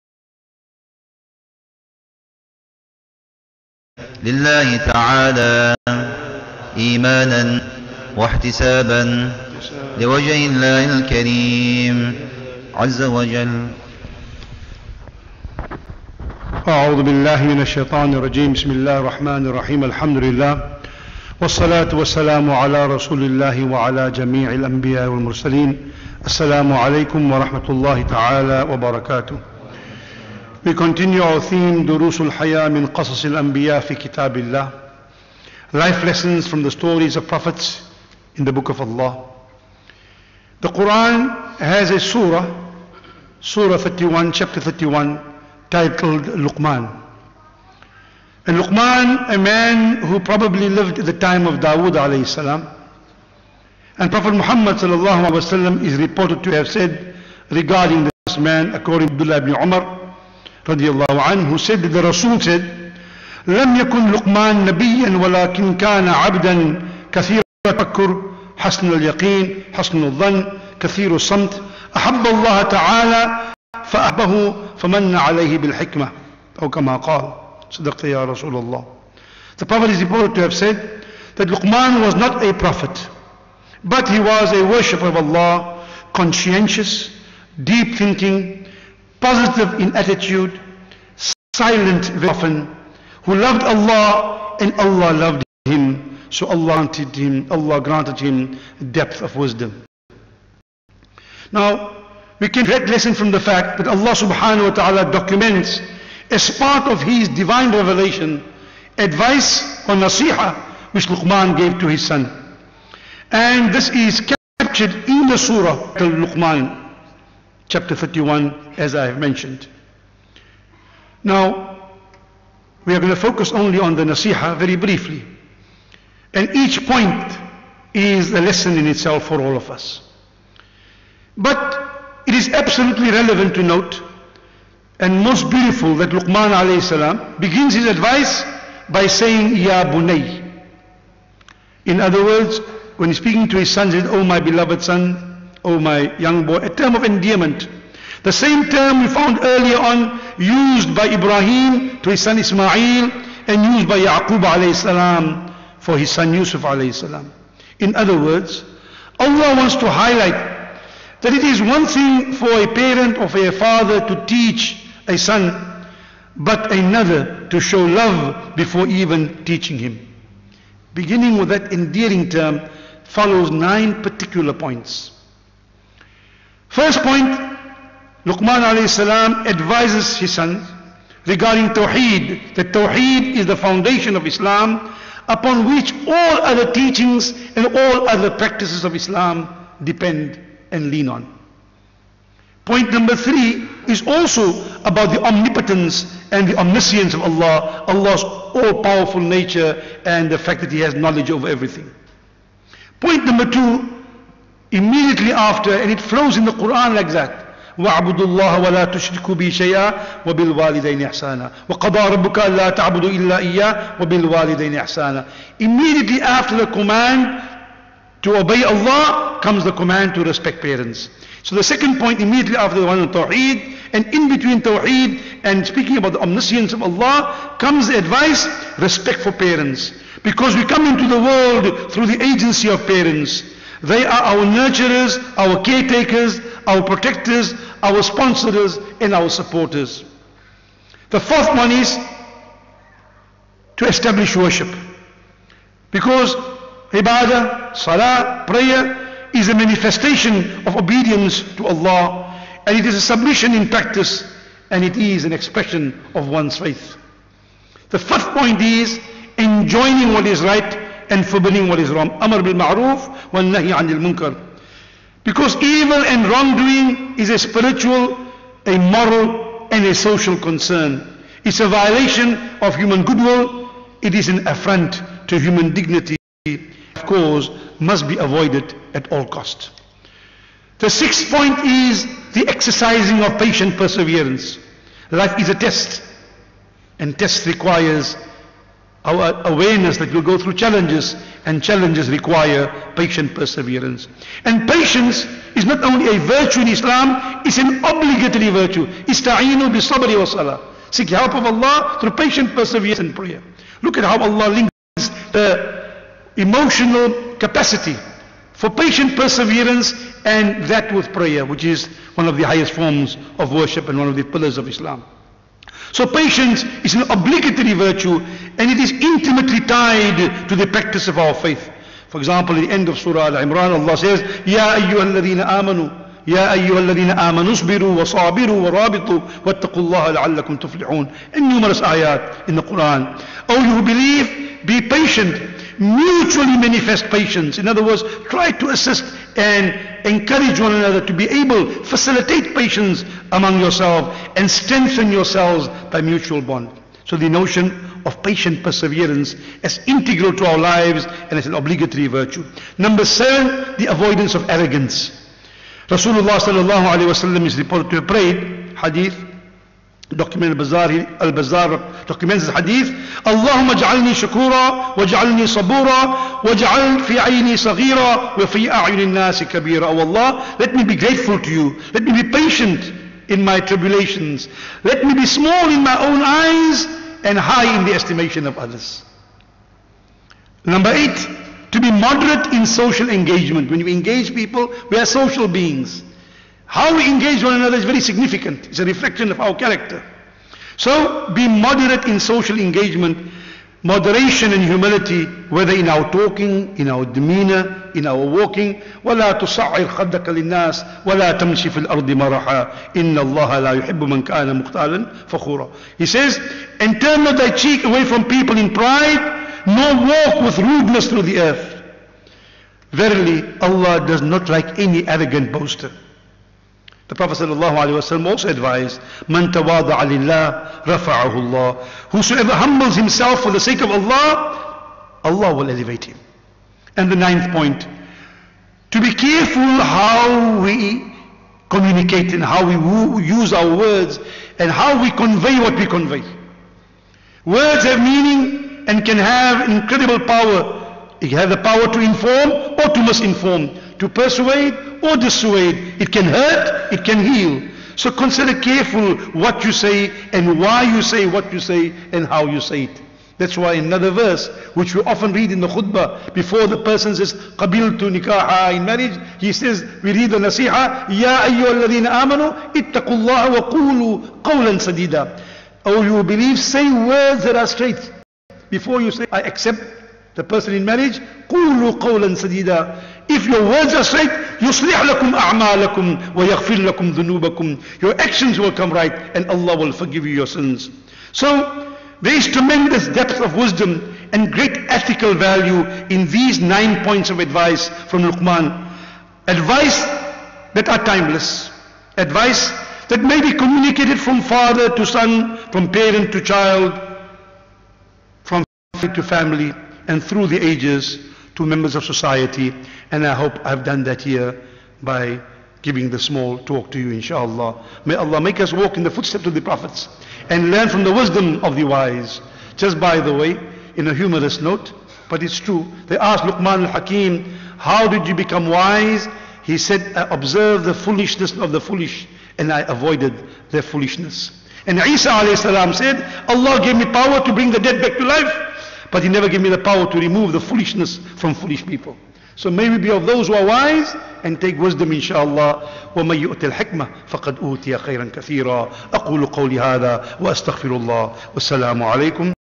Sh Sadullah Khan " Life Lessons from the Stories of the Prophets"
Masjid Al Furqaan LIVE Taraweeh Lecture